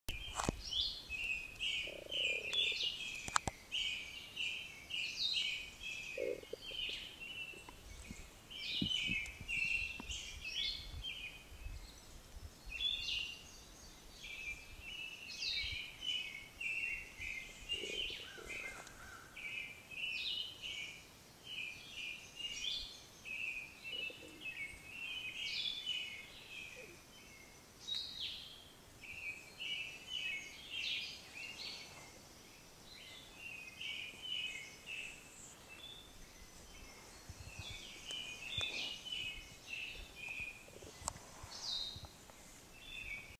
Evening at Sheep Pen Gap camp area
Listening to some bird songs as night falls at Sheep Pen Gap on Tuesday night.